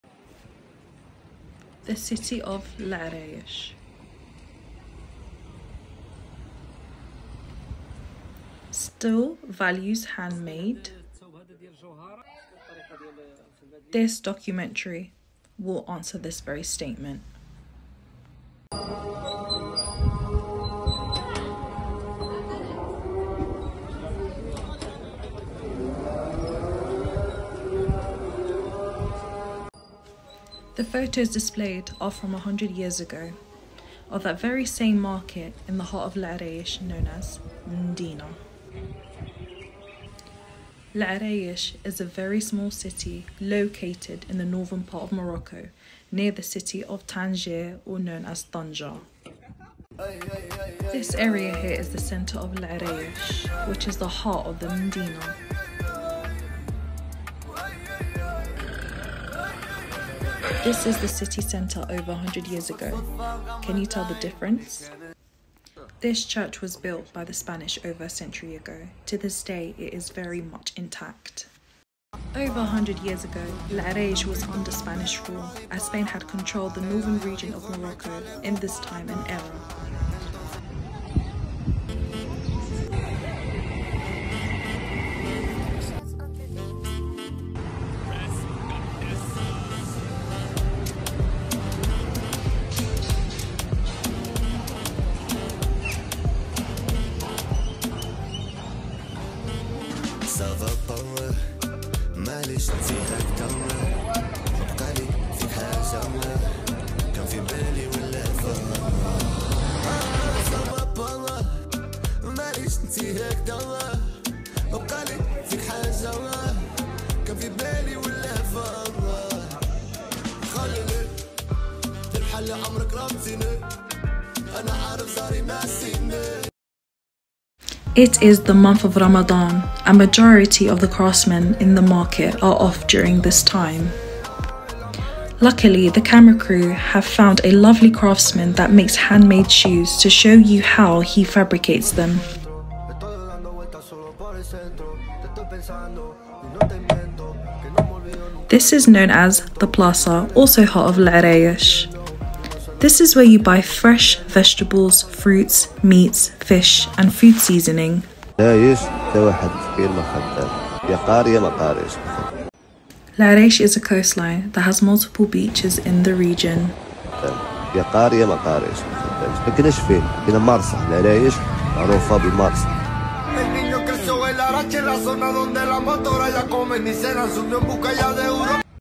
The city of Larache: still values handmade ? (Small snippet, full documentary will be posted soon.
This documentary is about if Larache has become more modernised or still holds the values and traditions till this day from 50-100+ years ago.

The example used, if Larache still holds the values and traditions from many years ago, is the Moroccan dejelleba. Alongside the landscape of the city and other handmade crafts.

The Moroccan artists used: Mocci, Morad and Stormy. The photos of Larache 100 years old are from Sergio Barce blog.